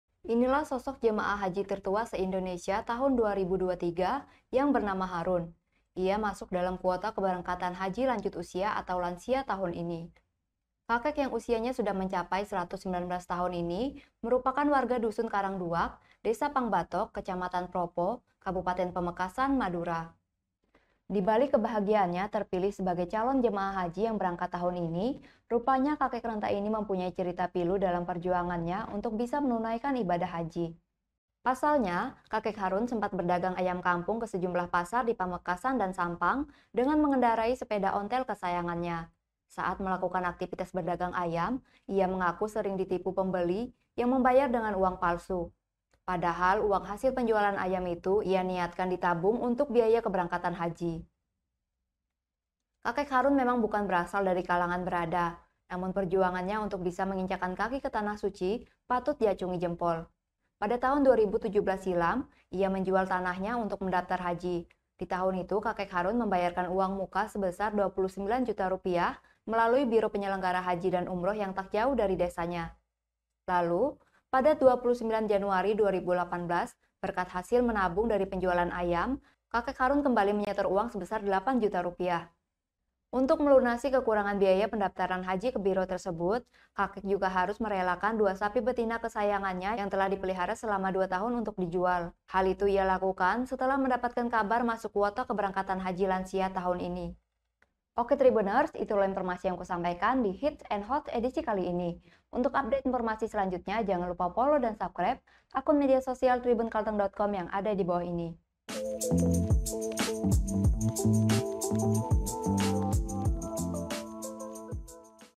Kisah Pilu Jemaah Haji Tertua Di Indonesia Berusia 119 Tahun
inilah sosok jemaah haji tertua di Indonesia yang bernama Harun. Niatnya pergi ke Arab Saudi untuk menunaikan ibadah haji akan terwujud tahun ini.

Harun berasal dari Pamekasan, Madura. Usianya kini sudah mencapai 119 tahun.

Harun, warga Dusun Karang Duak, Desa Pangbatok, Kecamatan Proppo, Kabupaten Pamekasan, Madura jadi jemaah haji lanjut usia (lansia) tertua se-Indonesia tahun 2023 ini.

Disamping usianya yang sudah renta, rupanya Kakek Harun menyimpan cerita pilu dalam perjuangannya untuk bisa menunaikan ibadah haji.